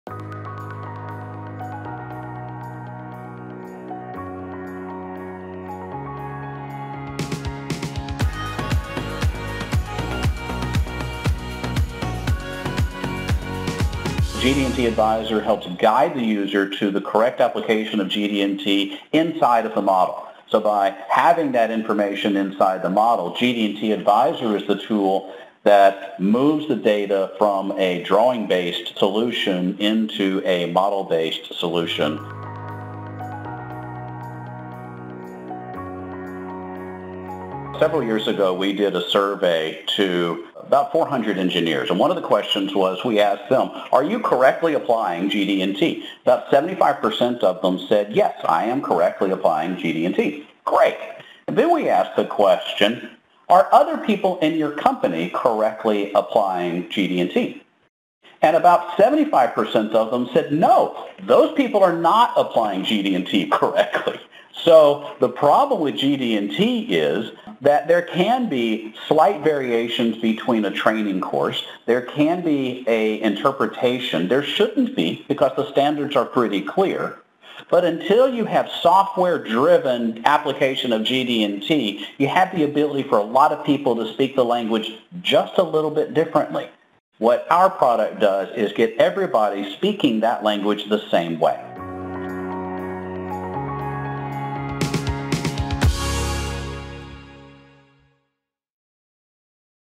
Why did Creo 4 include GD&T from Sigmetrix?
Chris Wilkes, Sigmetrix, explains why some people aren't as good at the language of GD&T as they might think.